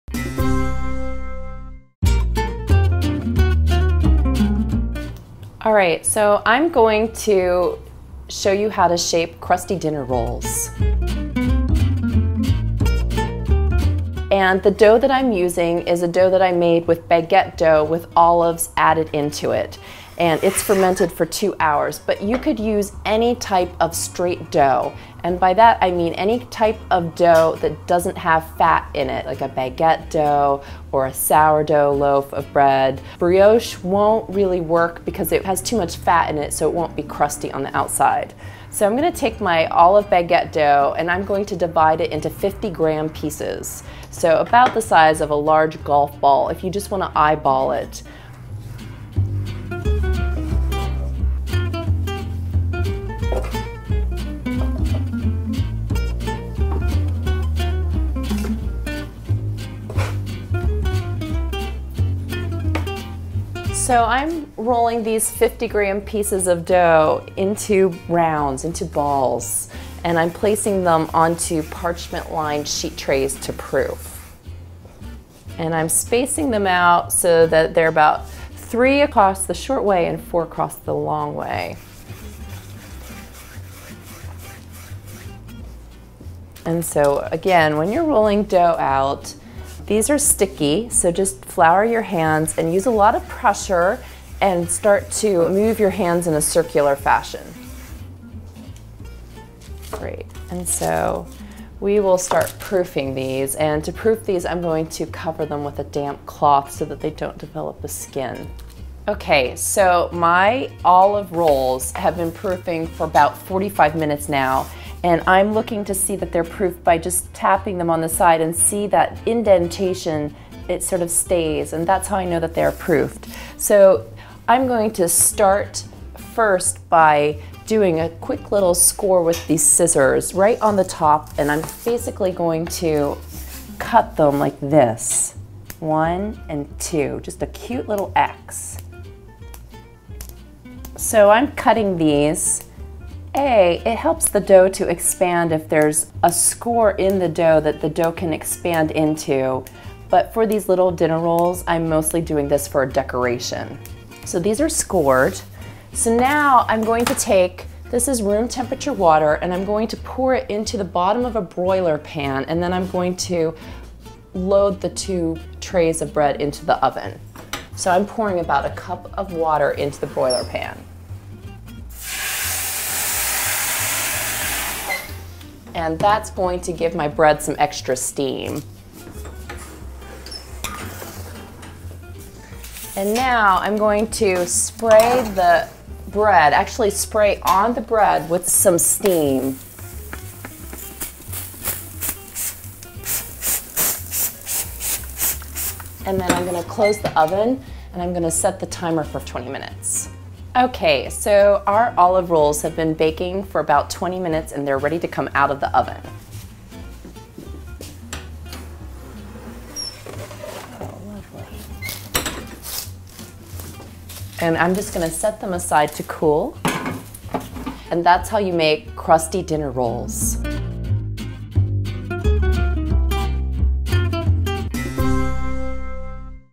How to Make Crusty Olive Dinner Rolls | Make Bread
RECIPE: 470 g all-purpose flour, 285 g room temp. water, 12 g sea salt, 3 g fresh yeast, 235 g poolish, 250 g. olives. Combine ingred. except salt and yeast in bowl; stir w. hands. Let dough rest w. salt on top for 30 min. Transfer dough to work surface; knead in yeast and salt til dough passes windowpane test. Knead 250 g. olives into baguette dough. Ferment 2 hours, punch down after 1st hour. Divide into 40g pieces and place onto parchment-lined baking sheet. Proof 1 hour and then bake with steam at 500F, lowering temp to 450F after 5 minutes. Bake about 20 minutes, until hollow to tap.

All right, so I'm going to show you how to shape crusty dinner rolls.The dough that I'm using is the dough that I made with baguette dough with olives added into it. It's fermented for two hours, but you could use any type of straight dough. By that I mean any dough that doesn't have fat in it, like a baguette dough or a sourdough loaf of bread. Brioche won't really work, because it has too much fat in it so it won't be crusty on the outside.

So I'm going to take my olive baguette dough and I'm going to divide it into 50 gram pieces, so about the size of a large golf ball, if you just want to eyeball it. I'm rolling these 50 gram pieces of dough into rounds, into balls and I'm placing them on to parchment lined sheet trays to proof. I'm spacing them out, so that they're about three across the short way and four across the long way.

Again, when you're rolling dough out, these are sticky so just flour your hands and use a lot of pressure and start to move your hands in a circular fashion. Great, and so we'll start proofing these and to proof these I'm going to cover them with a damp cloth, so that they don't develop a skin. So my olive rolls have been proofing for about 45 minutes now, and I'm looking to see that they're proofed by just tapping them on the side, and see hat indentation, it sort of stays and that's how I know that they are proofed.

I'm going to start first by doing a quick little score with these scissors, right on the top and I'm basically, going to cut them like this one and two, just a cute little X. So I'm cutting these A-- it helps the dough to expand if there's a score in the dough, that the dough can expand into. But for these little dinner rolls I'm mostly doing this for decoration, so these are scored.

Now I'm going to take this is warm temperature water and I'm going to pour it into the bottom of a broiler pan and then I'm going to  load the two trays of bread into the oven. So I'm pouring about a cup of water into the broiler pan and that's going to give my bread some extra steam.

Now I'm going to spray the bread, actually spray on the bread with some steam. And then I'm going to close the oven and I'm going to set the timer for 20 minutes. Okay, so our olive rolls have been baking for about 20 minutes and they're ready to come out of the oven. I'm just going to set them aside to cool, and that's how you make crusty dinner rolls.